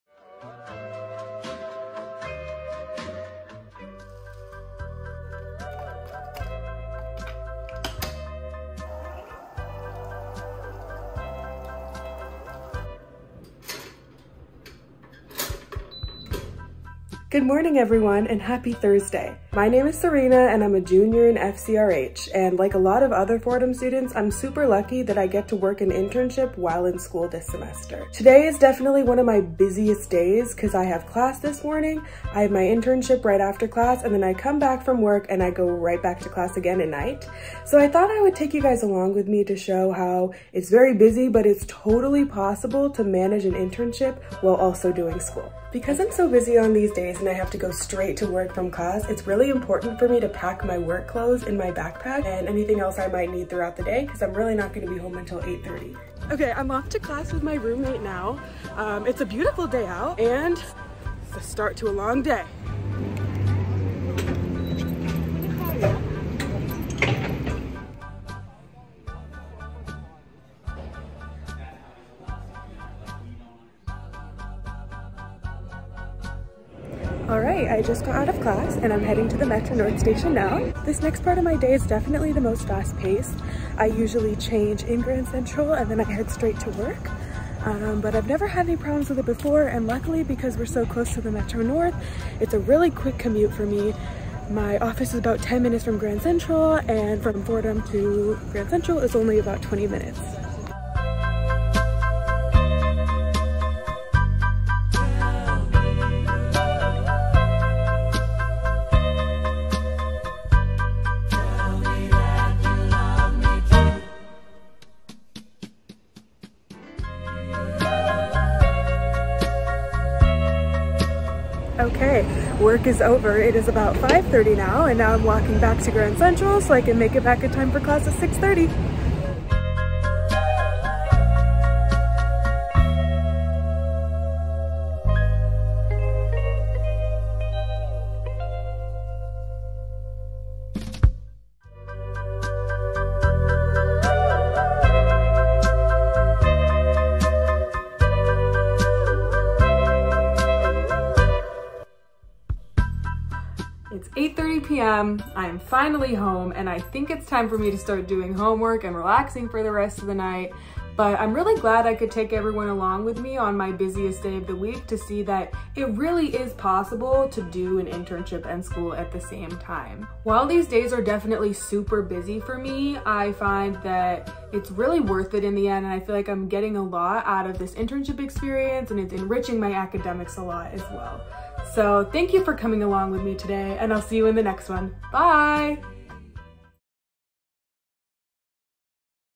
From the Ram Cam: Ep. 16 | Balancing an Internship
Balancing an internship and going to school full-time requires time management, flexibility, and a little creativity. Sarina S., FCRH '23, shows us how she does it.